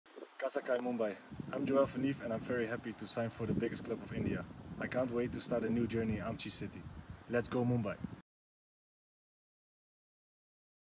Yoell has a message for you! | Mumbai City FC
🔊 Yoëll's becoming a proper Mumbaikar already, and he's got a message for you.. 🩵🧡